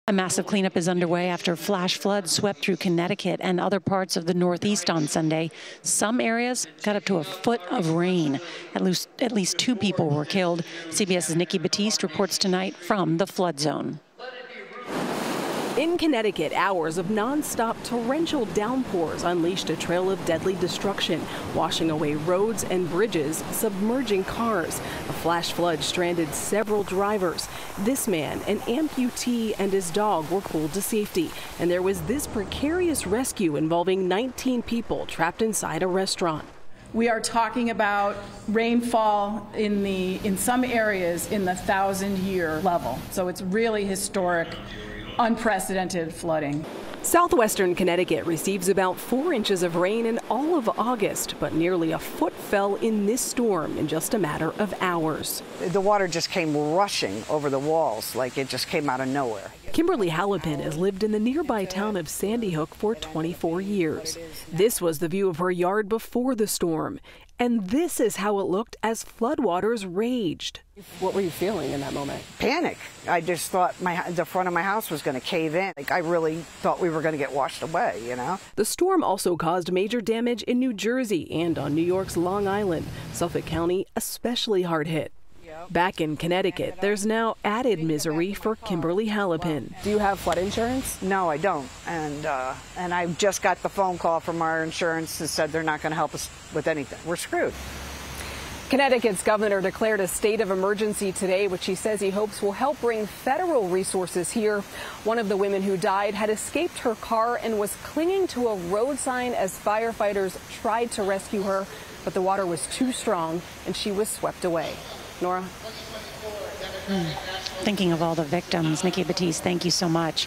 At least 2 killed as flash floods hit Northeast
At least two people have died as a result of flash flooding that hit Connecticut and other parts of the Northeast. Parts of New Jersey and New York's Long Island were also hit hard. Nikki Battiste reports.